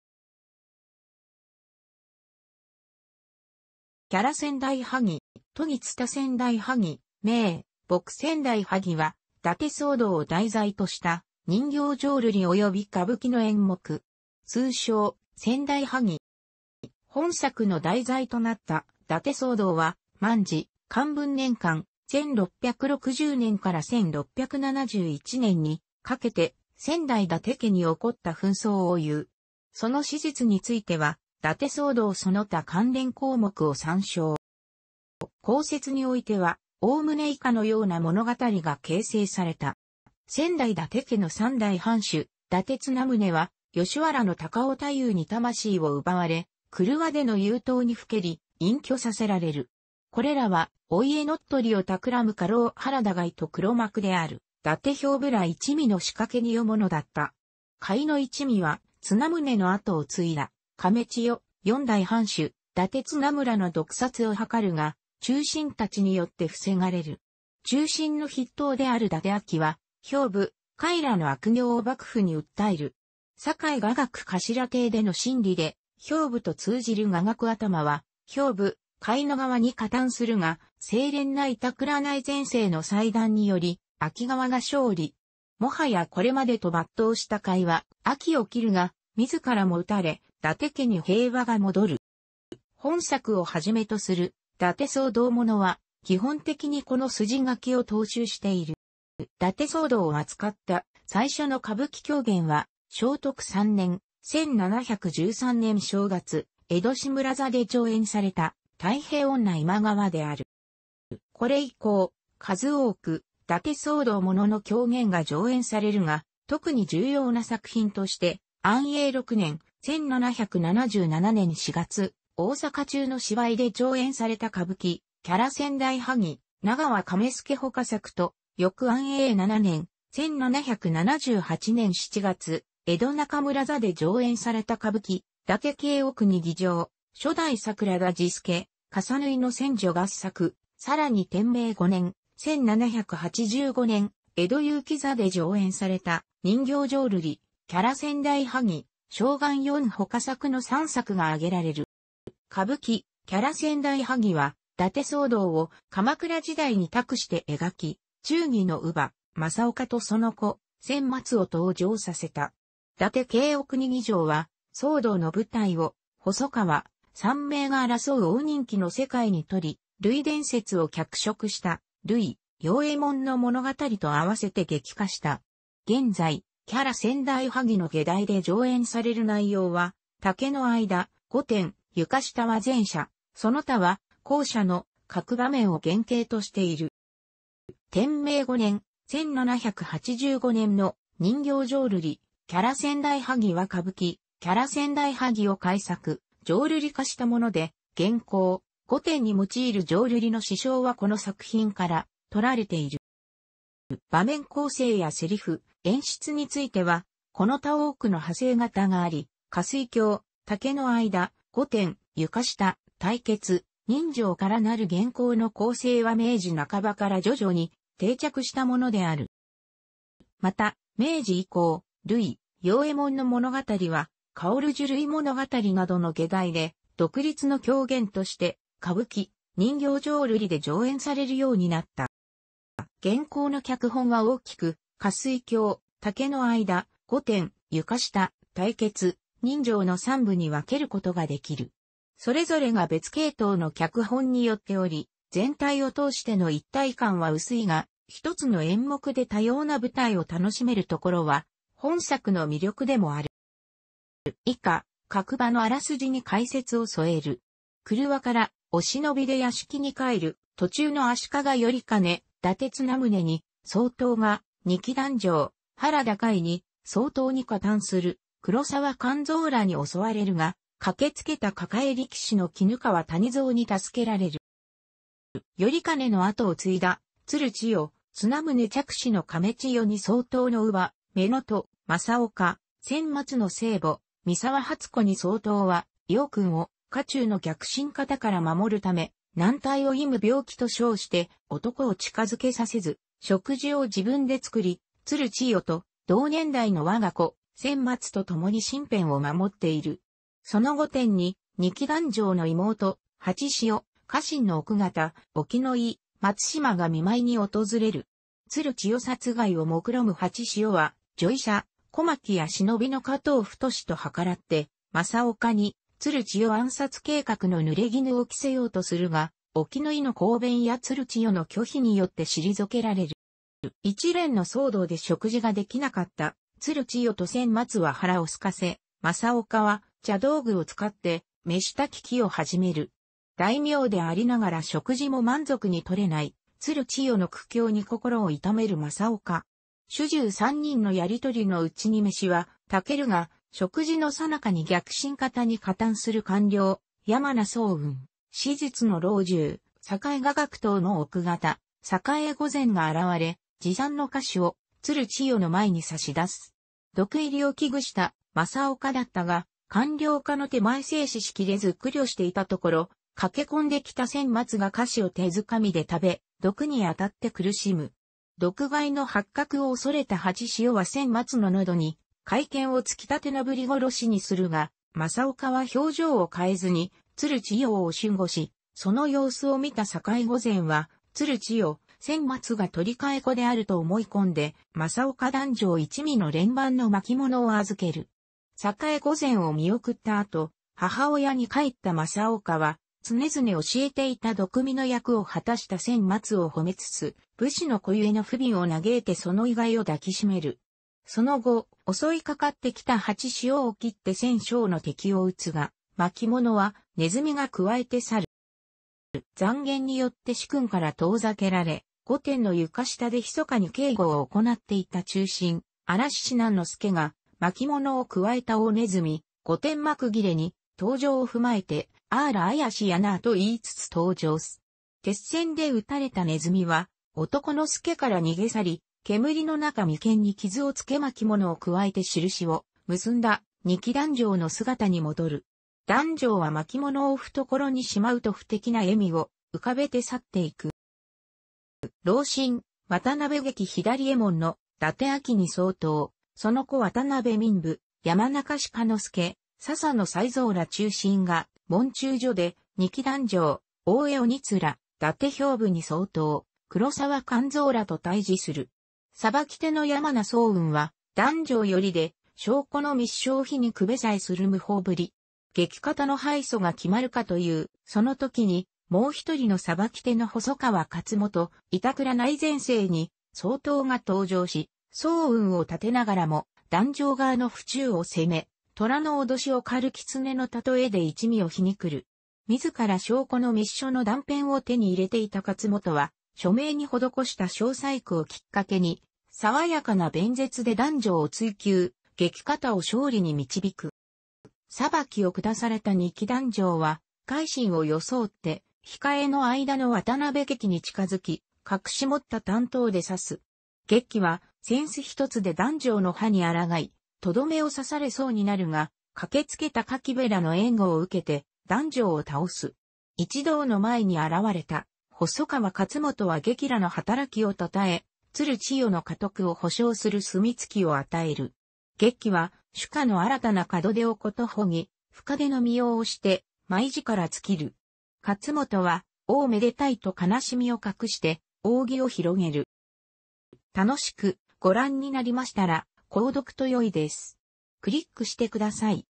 伽羅先代萩
『伽蘿先代萩』（めいぼく せんだいはぎ）は、伊達騒動を題材とした人形浄瑠璃および歌舞伎の演目。通称「先代萩」。

本作の題材となった伊達騒動は、万治・寛文年間、1660年から1671年にかけて仙台伊達家に起こった紛争をいう。その史実については、伊達騒動その他関連項目を参照。

巷説においては、おおむね以下のような物語が形成された。

仙台伊達家の3代藩主・伊達綱宗は吉原の高尾太夫に魂を奪われ、廓での遊蕩にふけり、隠居させられる。これらはお家乗っ取りをたくらむ家老原田甲斐と黒幕である伊達兵部ら一味の仕掛けによるものだった。甲斐一味は綱宗の後を継いだ亀千代（4代藩主・伊達綱村）の毒殺を図るが、忠臣たちによって防がれる。忠臣の筆頭である伊達安芸は兵部・甲斐らの悪行を幕府に訴える。酒井雅楽頭邸での審理で、兵部と通じる雅楽頭は兵部・甲斐側に加担するが、清廉な板倉内膳正の裁断により安芸側が勝利。もはやこれまでと抜刀した甲斐は安芸を斬るが自らも討たれ、伊達家に平和が戻る。

本作をはじめとする伊達騒動ものは基本的にこの筋書きを踏襲している。

伊達騒動を扱った最初の歌舞伎狂言は、正徳3年（1713年）正月、江戸市村座で上演された『泰平女今川』である。

これ以降、数多く伊達騒動ものの狂言が上演されるが、特に重要な作品として、安永6年（1777年）4月、大坂中の芝居で上演された歌舞伎『伽羅先代萩』（奈河亀輔ほか作）と、翌安永7年（1778年）7月、江戸中村座で上演された歌舞伎『伊達競阿国戯場』（初代桜田治助・笠縫専助合作）、さらに天明5年（1785年）、江戸結城座で上演された人形浄瑠璃『伽羅先代萩』（松貫四ほか作）の3作が挙げられる。

歌舞伎『伽羅先代萩』は、伊達騒動を鎌倉時代に託して描き、忠義の乳母・政岡とその子・千松を登場させた。『伊達競阿国戯場』は、騒動の舞台を細川・山名が争う応仁記の世界にとり、累伝説を脚色した累・与右衛門の物語と併せて劇化した。現在『伽羅先代萩』の外題で上演される内容は、「竹の間」「御殿」「床下」は前者、その他は後者の各場面を原型としている。

天明5年（1785年）の人形浄瑠璃『伽羅先代萩』は歌舞伎『伽羅先代萩』を改作・浄瑠璃化したもので、現行「御殿」に用いる浄瑠璃の詞章はこの作品から取られている。

場面構成や科白・演出についてはこの他多くの派生形があり、「花水橋」「竹の間」「御殿」「床下」「対決」「刃傷」からなる現行の構成は明治半ばから徐々に定着したものである。

また明治以降、累・与右衛門の物語は『薫樹累物語』などの外題で、独立の狂言として歌舞伎・人形浄瑠璃で上演されるようになった。

現行の脚本は大きく「花水橋」「竹の間・御殿・床下」「対決・刃傷」の3部に分けることができる。それぞれが別系統の脚本によっており、全体をとおしての一体感は薄いが、一つの演目で多様な舞台を楽しめるところは本作の魅力でもある。

以下、各場のあらすじに解説を添える。
廓からお忍びで屋敷に帰る途中の足利頼兼（伊達綱宗に相当）が、仁木弾正（原田甲斐に相当）に加担する黒沢官蔵らに襲われるが、駆けつけた抱え力士の絹川谷蔵に助けられる。

頼兼の跡を継いだ鶴千代（綱宗嫡子の亀千代に相当）の乳母（めのと）・政岡（千松の生母・三沢初子に相当）は、幼君を家中の逆臣方から守るため、男体を忌む病気と称して男を近づけさせず、食事を自分で作り、鶴千代と同年代の我が子・千松とともに身辺を守っている。その御殿に、仁木弾正の妹・八汐、家臣の奥方・沖の井、松島が見舞いに訪れる。鶴千代殺害をもくろむ八汐は、女医者・小槙や忍びの嘉藤太とはからって政岡に鶴千代暗殺計画の濡れ衣を着せようとするが、沖の井の抗弁や鶴千代の拒否によって退けられる。

一連の騒動で食事ができなかった鶴千代と千松は腹をすかせ、政岡は茶道具を使って飯焚きを始める。大名でありながら食事も満足に取れない鶴千代の苦境に心を痛める政岡。主従3人のやりとりのうちに飯は炊けるが、食事のさなかに逆臣方に加担する管領・山名宗全（史実の老中・酒井雅楽頭）の奥方・栄御前が現われ、持参の菓子を鶴千代の前に差し出す。毒入りを危惧した政岡だったが、管領家の手前制止しきれず苦慮していたところ、駆け込んで来た千松が菓子を手づかみで食べ、毒にあたって苦しむ。毒害の発覚を恐れた八汐は千松ののどに懐剣を突き立てなぶり殺しにするが、政岡は表情を変えずに鶴千代を守護し、その様子を見た栄御前は鶴千代・千松が取り替え子であ...